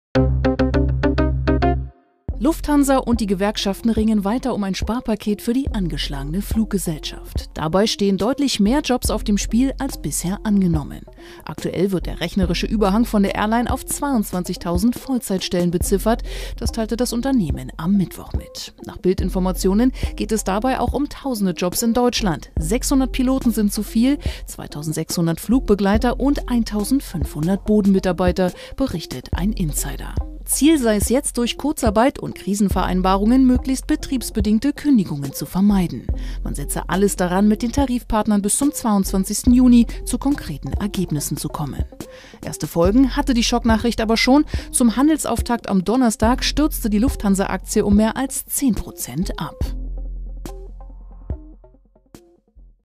So will Lufthansa die Kündigung von 22.000 Stellen verhindern
Die neuste Schocknachricht: Die Lufthansa hat deutlich mehr Personal zu viel an Bord als bisher bekannt - Tausende Stellen stehen auf der Kippe!